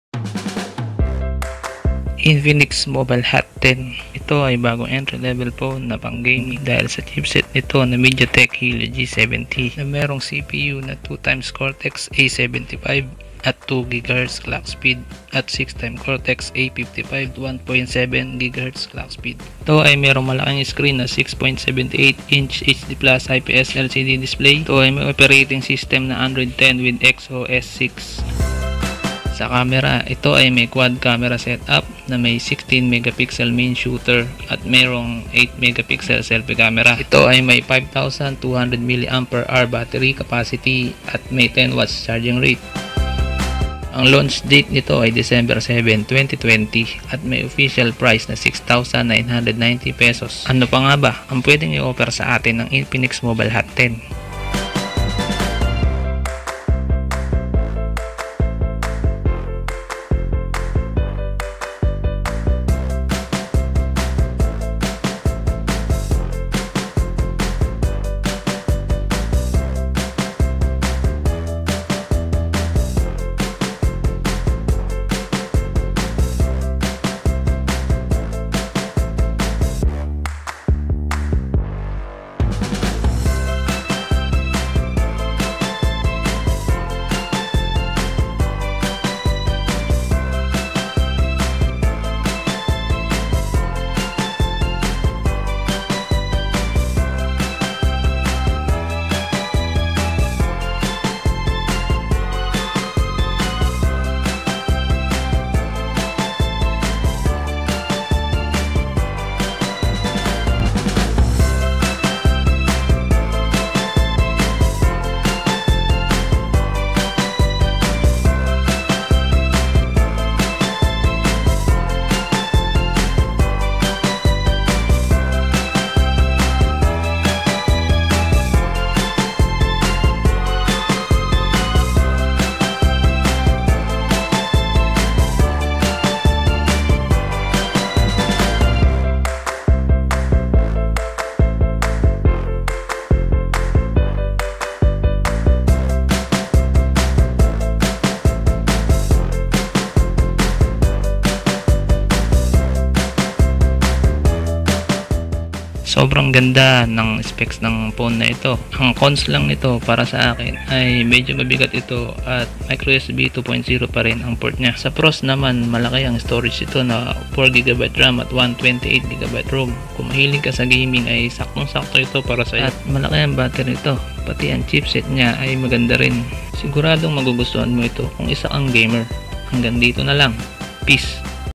Infinix Mobile Hot 10 Specs review| solid pang gaming
Ito Ang infinix Mobile Hot 10 Ang bagong entry level phone na pang gaming dahil naka Helio g70 ang chipset nito,malaki ang storage at battery,
sa video na Ito at nireview ko ang specs
at aking binahagi ang opinion ko about sa phone.